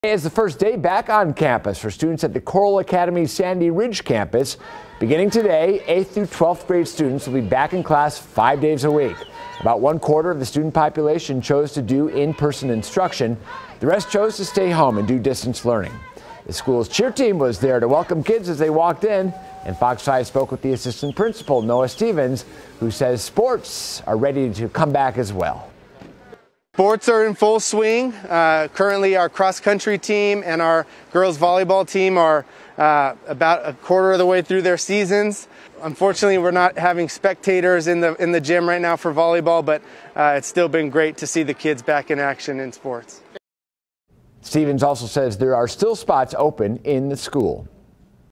Coral Academy welcomes back students to 5 days a week
Monday was the first day back on campus for students at the Coral Academy's Sandy Ridge campus.